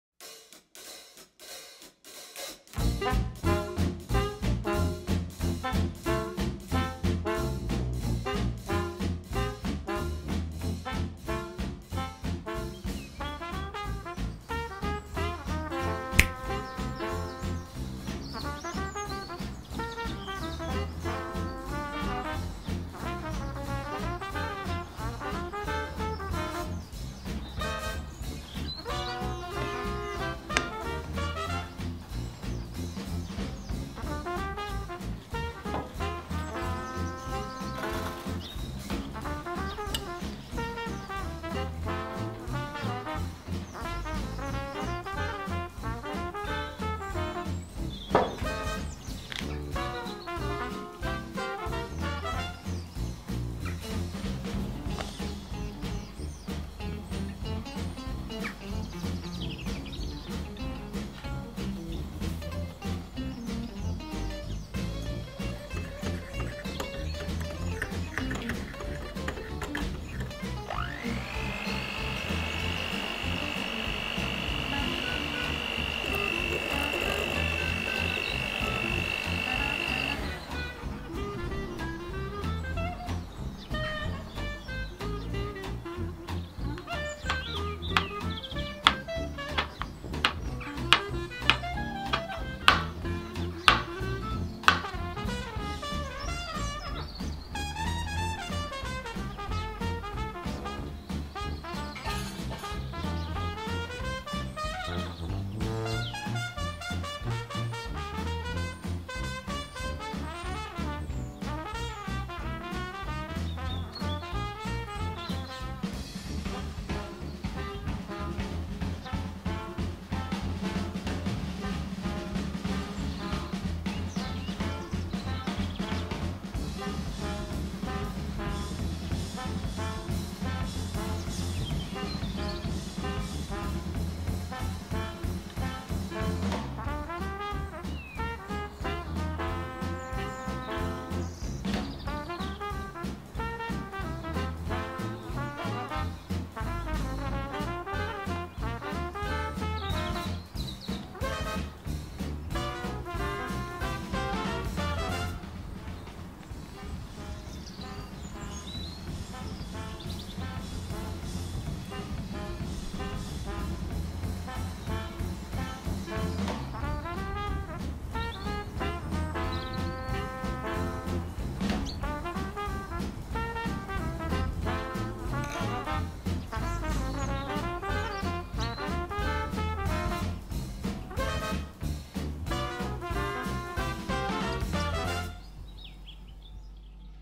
Creamiest Banoffee Pie Recipe! ألذ وصفة بانوفي باي! كيكة رائعة بدون فرن
How to Make Creamy Banana Banoffee Pie Cake! Super Delicious & Easy!
طريقة صنع البانوفي باي كيك أو فطيرة البنوفي بالموز